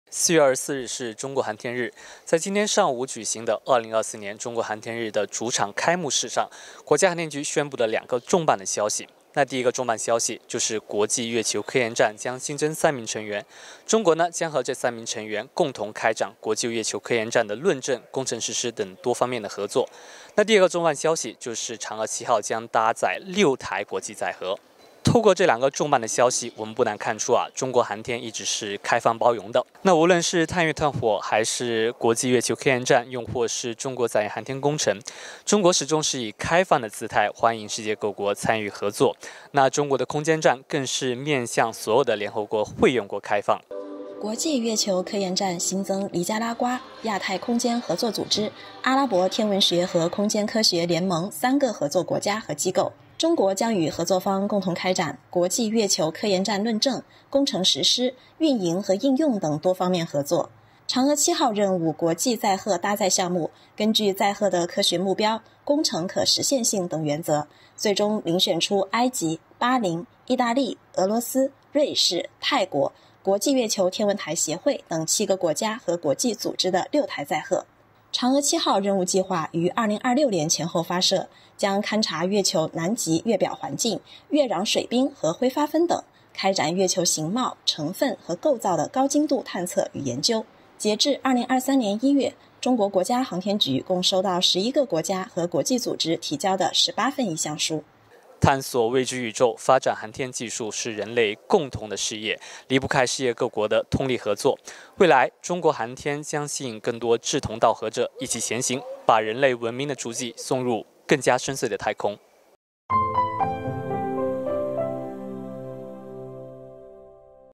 国际月球科研站再添3个成员 嫦娥七号将搭载6台国际载荷
4月24日，国家航天局在2024 年“中国航天日”主场活动开幕式上，现场发布有关国际月球科研站合作进展和嫦娥七号任务国际载荷搭载项目的最新消息。

国际月球科研站新增尼加拉瓜、亚太空间合作组织、阿拉伯天文学和空间科学联盟3个合作国家和机构。中国将与合作方共同开展国际月球科研站论证、工程实施、运营和应用等多方面合作。嫦娥七号任务国际载荷搭载项目，根据载荷的科学目标、工程可实现性等原则，最终遴选出埃及、巴林、意大利、俄罗斯、瑞士、泰国、国际月球天文台协会等7个国家和国际组织的6台载荷。